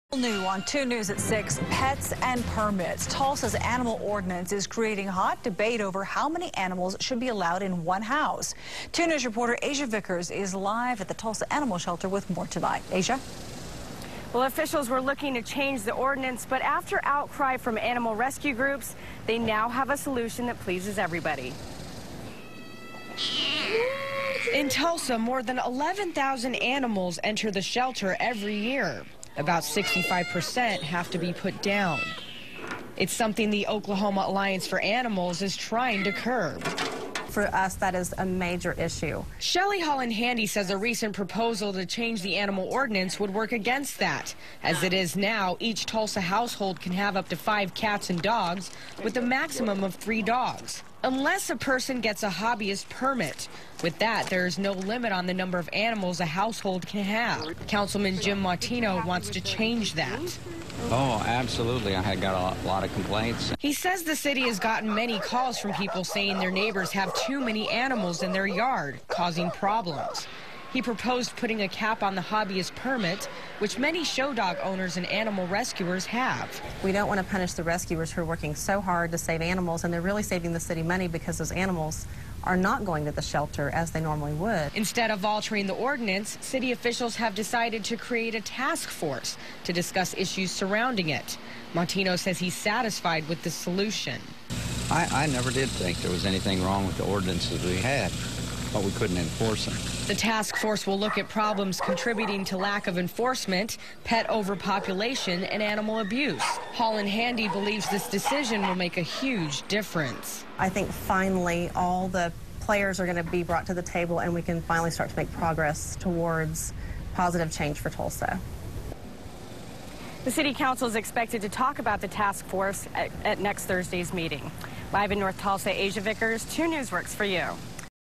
Tulsa's animal ordinance stirs debate
Officials were looking to change the animal ordinance, but after outcry from animal rescue groups there's now a solution that makes everyone happy.